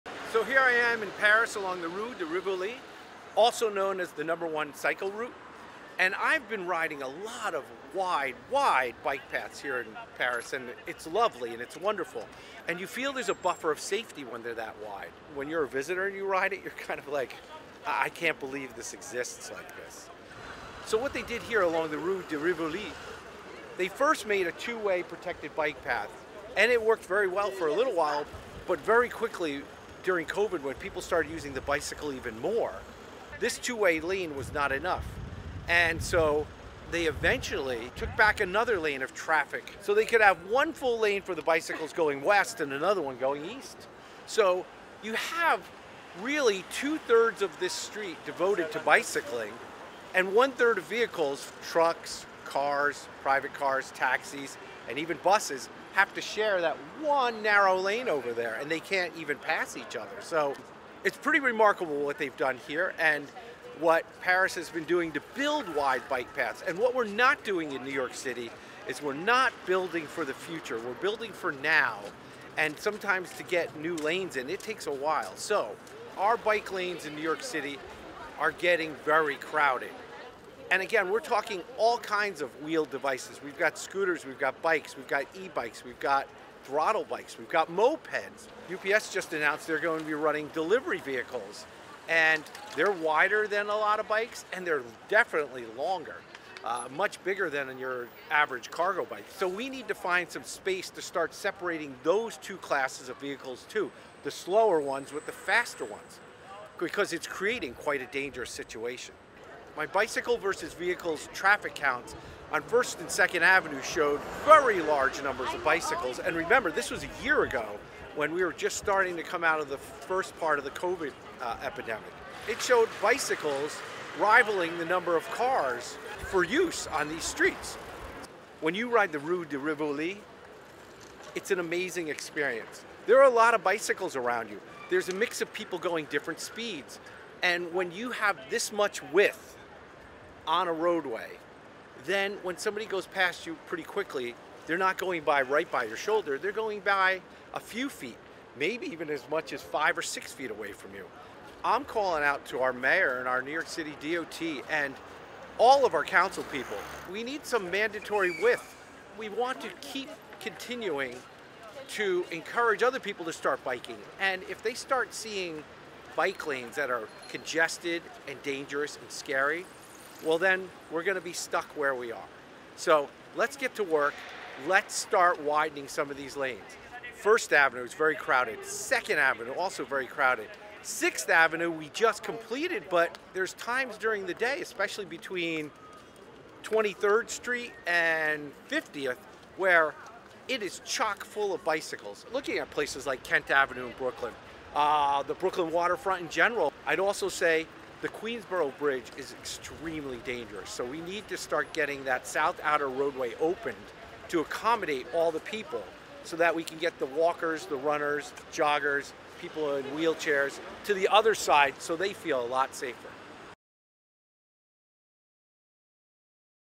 NYC's Bike Lanes are too narrow, see how Paris widened the Rue de Rivoli
I just got back from Paris to bike around (many Streetfilms coming) and it is so much fun to ride on some of their newest super-wide bike lanes. The comfort level is amazing - none so much as the Rue de Rivoli which is now 2/3rds dedicated to the movement of all sorts of wheeled-devices including bicycles of course.

In NYC we have some bike lanes that are overcrowded (to be kind) and outright getting dangerous & scary to ride on (during rush hours). In the last year I conducted three bike counts over a 30 minute period which shows the number of bikes & other users to be nearly on par with the number of cars who have the majority of the roadway. See this article among many

So this is my pitch to NYC's elected leaders and the administration, let's take a lesson from Paris where they have been doing much more to make bike riders more comfortable - which of course leads to more people riding, fewer crashes and a reduction in pollution (and ultimately less car ownership).